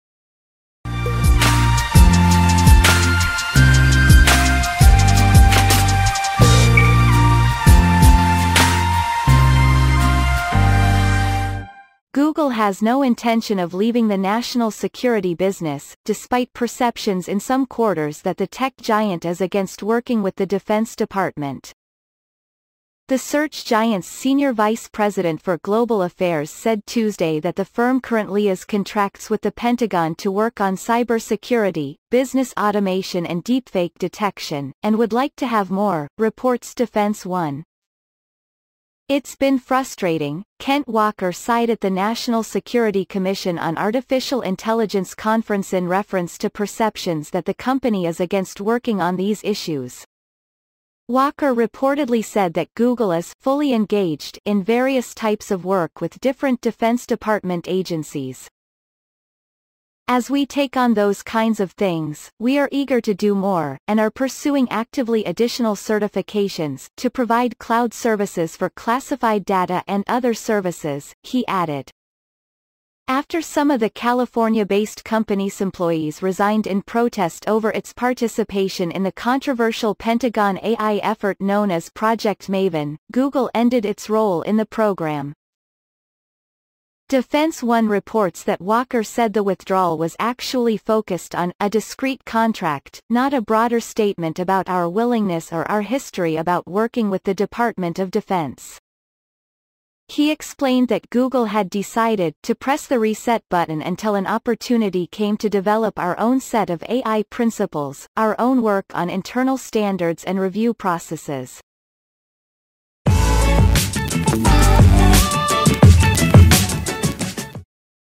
Google wants ‘more work’ with Defense Department, top exec says
Google has no intention of leaving the national security business, despite perceptions in some quarters that the tech giant is against working with the Defense Department.  The search giant's senior vice president for global affairs said Tuesday that the firm currentlyhas contracts with the Pentagon to work on cybersecurity, business automation and deepfake detection, and would like to have more, reports DefenseOne.  "It's been frustrating,"Kent Walker saidat the National Security Commission on Artificial Intelligence...

Reference from FoxNews Tech